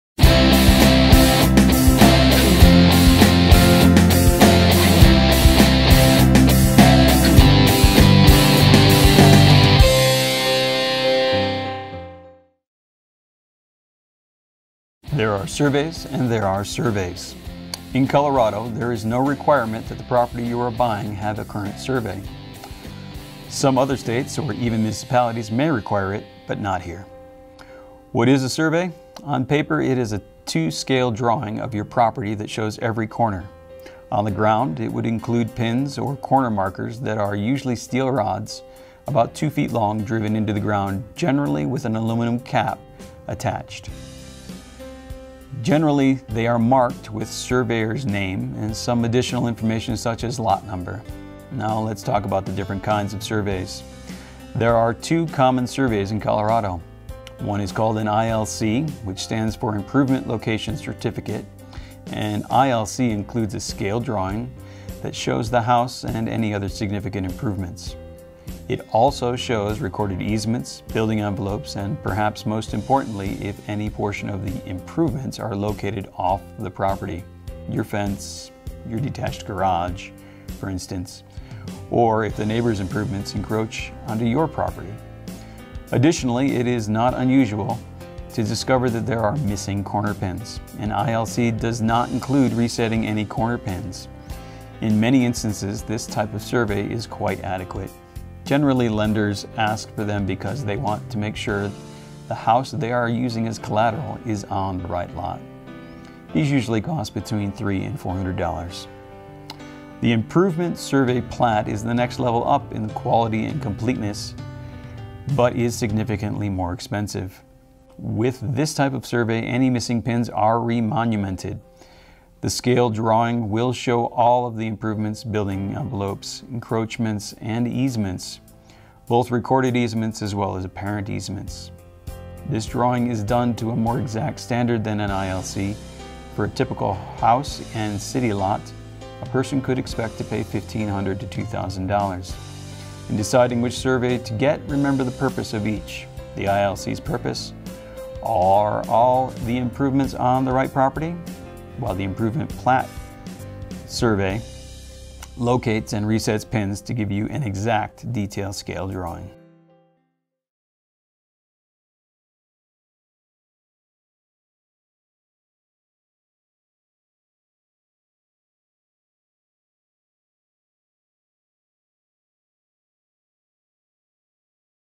Are Surveys Important?
This is part of our continuing effort to keep you, the Buyer informed about Durango Colorado real estate.
There are surveys and then there are things that look like surveys.  Which one will you be getting and which one do you need?